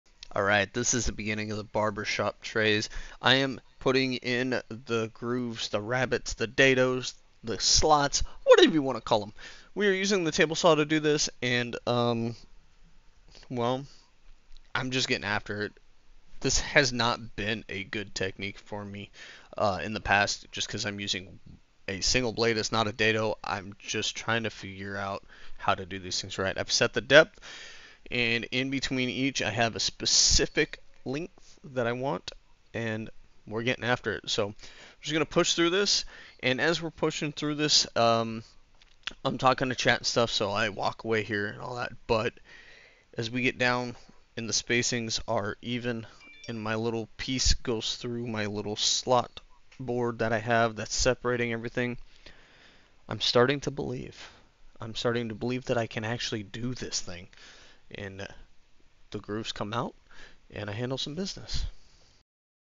Barber Trays Slot cutting
This is the process that I went through to make 3 personalized Barber/organization trays.